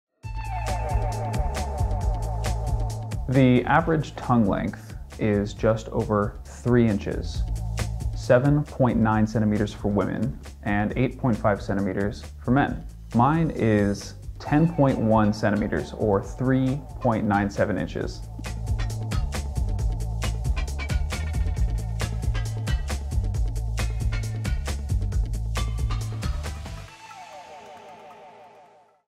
Longest tongue 👅 10.1 cm (3.97 in) belonging to Nick Stoeberl 🇺🇸
The longest tongue measures 10.1 cm (3.97 in) from its tip to the middle of the closed top lip and belongs to Nick Stoeberl (USA). It was verified in Salinas, California, USA, on 27 November 2012.